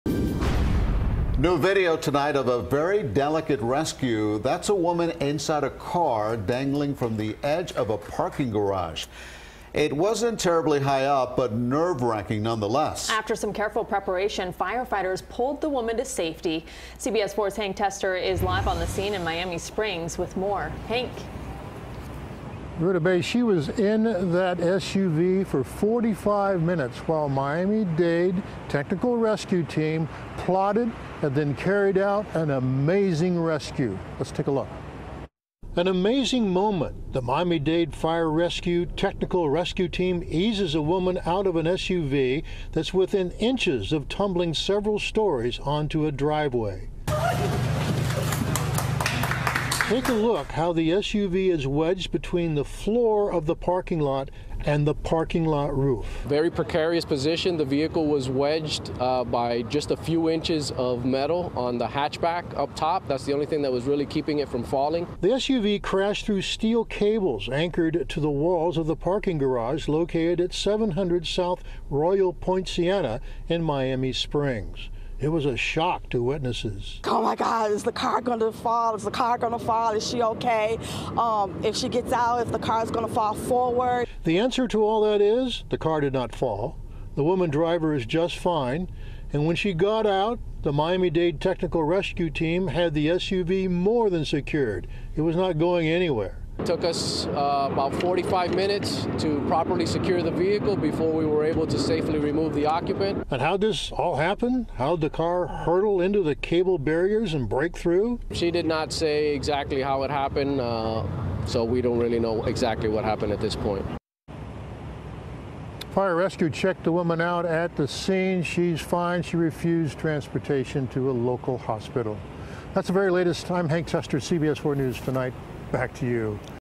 Rescue Workers Pull Woman From Dangling Car After Crash Through Parking Garage Wall
A woman was pulled from a dangling car on the side of a parking garage in Miami Springs on Thursday afternoon.